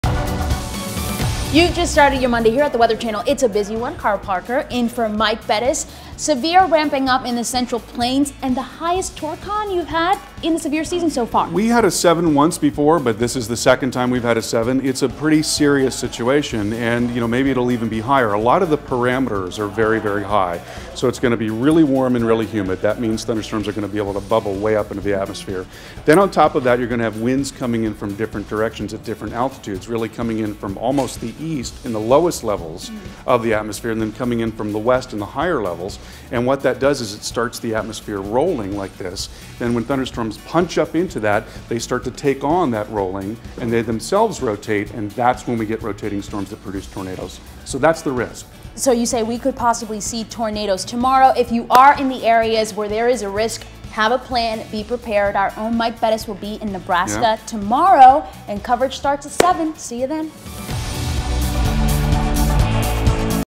Carl and Anaridis Talk Severe
Carl Parker and Anaridis Rodriguez discuss severe weather.